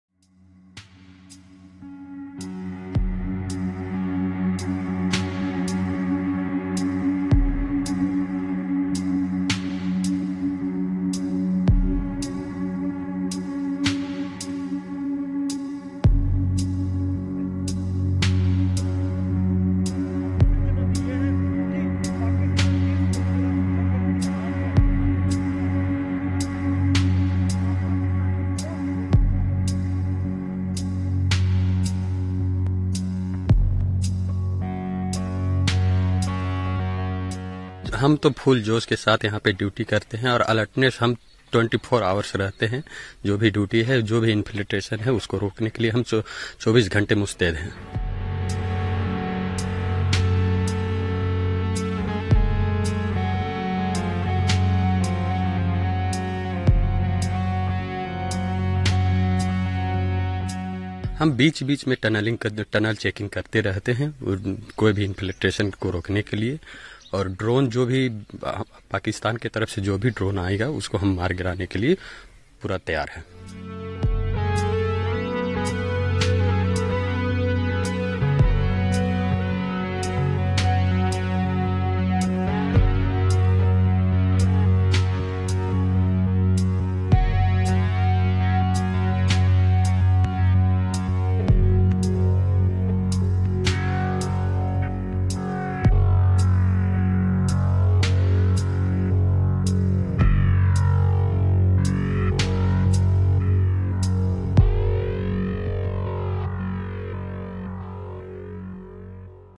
BSF Jawans intensify patrolling along international border in Jammu
Get All Latest Updates Here:
IndiaTV News in Hindi The Border Security Force (BSF) soldiers are on high alert at the international border with Pakistan in Jammu region.